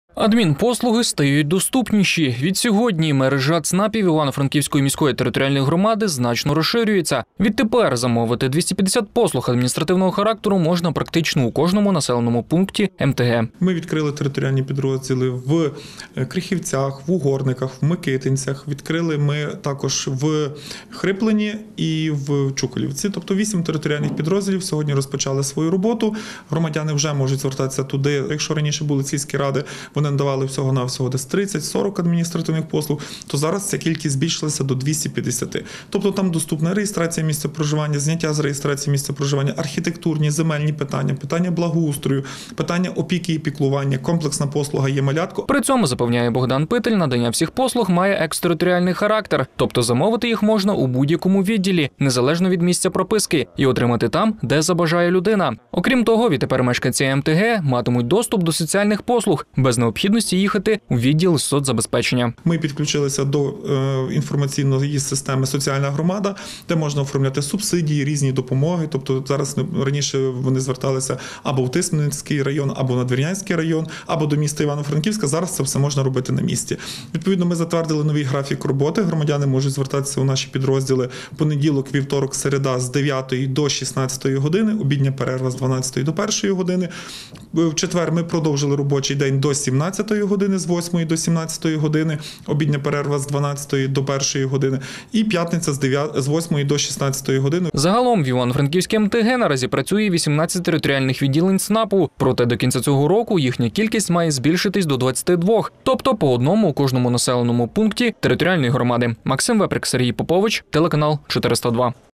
В Івано-Франківській МТГ розпачали роботу 8 нових відділень ЦНАПу
Тепер замовити 250 послуг адміністративного характеру можна практично у кожному населеному пункті МТГ.